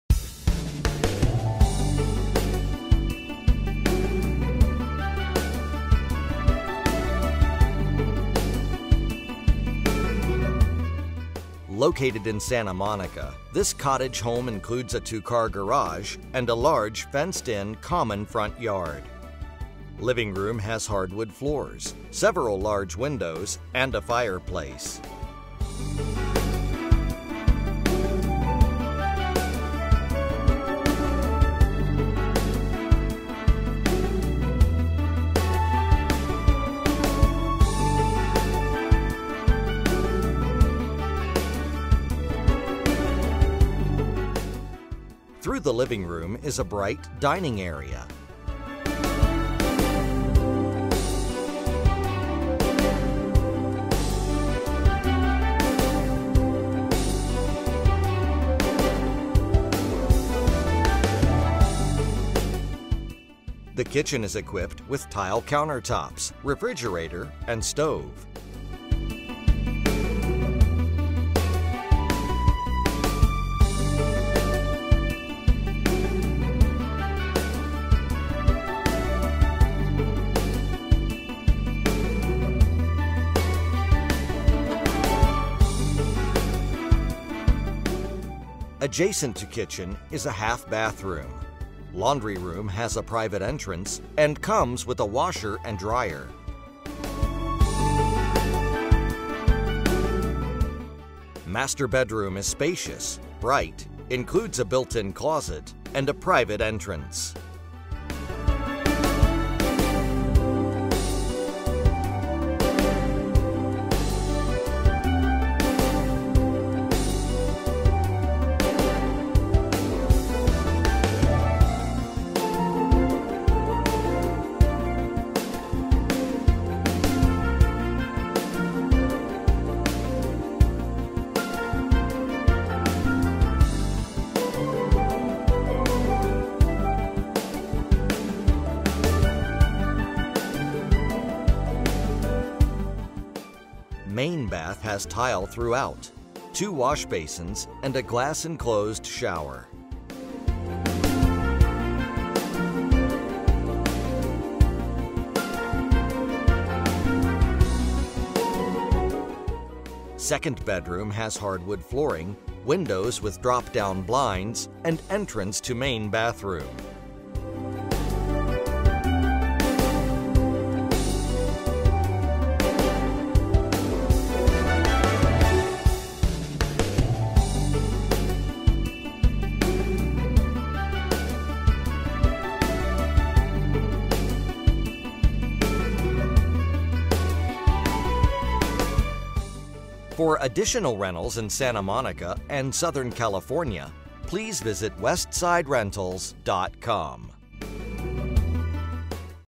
West Hollywood Apartments
Contact WestsideRentals.com at - Located in Santa Monica, this cottage home includes a 2-car garage, and a large, fenced in, common front yard.   Living room has hardwood floors, several large windows, and a fireplace.  Through the living room is a bright, dining area. The Kitchen is equipped with tile countertops, refrigerator, and stove. Adjacent to kitchen is a half bathroom. Laundry room has a private entrance and comes with a washer and dryer.   Master Bedroom is spacious, bright, includes a built in closet, and a private entrance.   Main bath has tile throughout, 2 wash basins, and a glass enclosed shower.   Second bedroom has hardwood flooring, windows with drop down blinds, and entrance to main bathroom.   For additional rentals in Santa Monica and Southern California, please visit WestsideRentals.com!